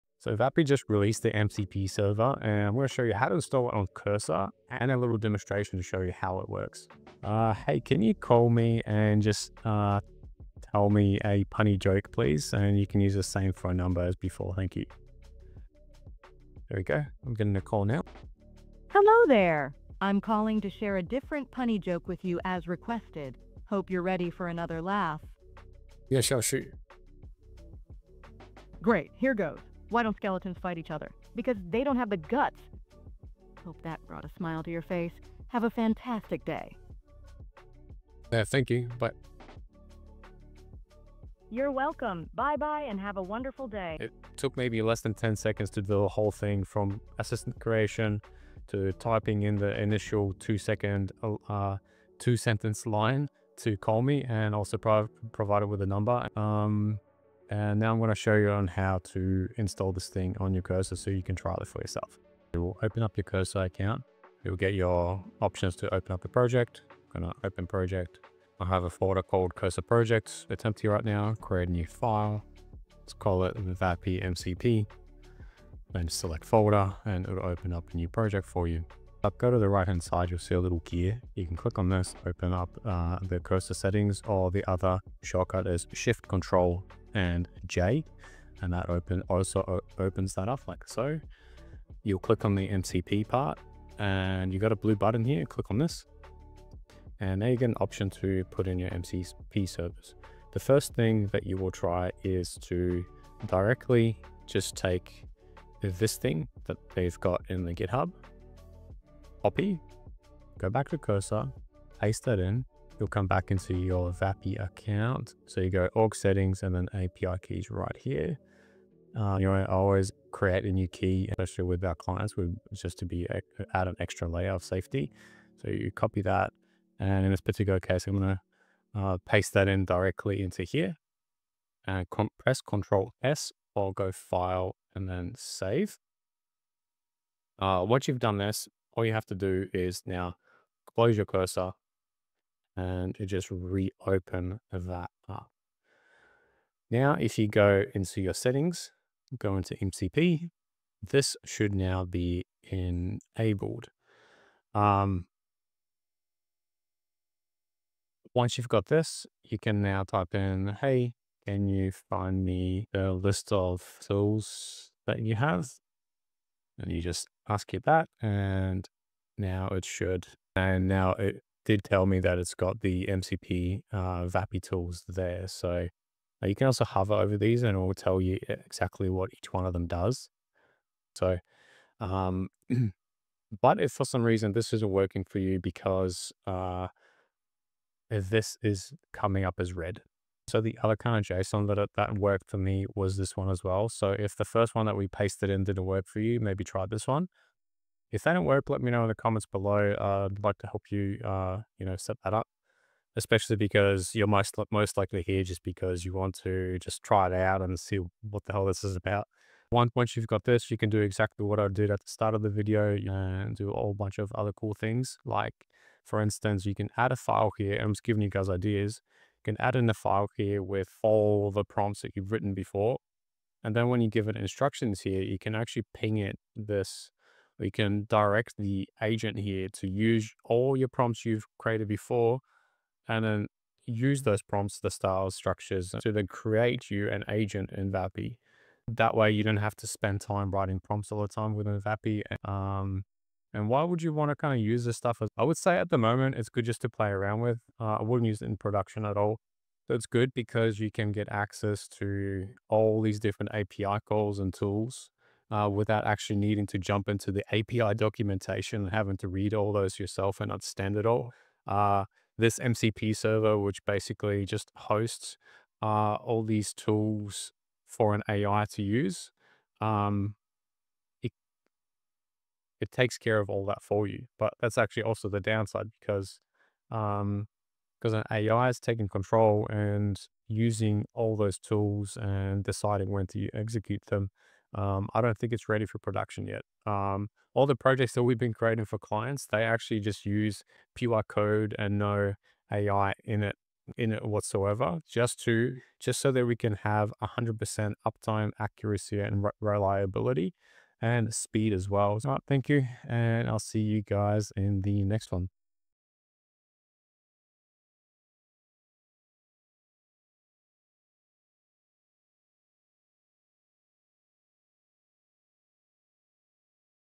Use Vapi MCP on Cursor - Installation Guide and Demo - Its awesome!
Vapi just released their MCP server and here I'll show you how to install it on your Cursor IDE and use it to create calls or build agents directly from within Cursor without needing to even be inside of your Vapi account.

Option 1:
  "mcpServers": {
    "vapi-mcp-server": {
      "command": "npx",
      "args": [
          "-y",
          "@vapi-ai/mcp-server"
      "env": {
        "VAPI_TOKEN": "Your key here"

Option 2:
  "mcpServers": {
    "vapi": {
      "command": "cmd",
      "args": [
        "/c",
        "npx",
        "-y",
        "@vapi-ai/mcp-server"